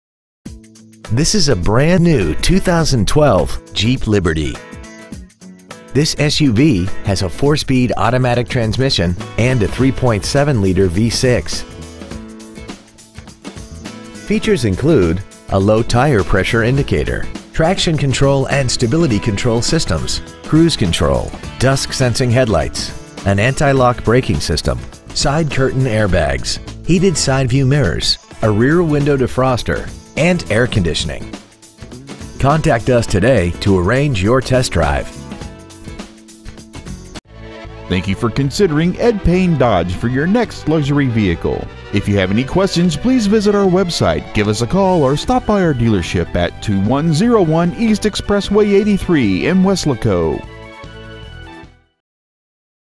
2012 Jeep Liberty Harlingen TX
Year : 2012
 Make : Jeep
 Model : Liberty
 Engine : 3.7L V6
 Trans . : Automatic 4 Speed
 Exterior : Mineral Gry Met

 Stock : W141755

 2101 E. Exp 83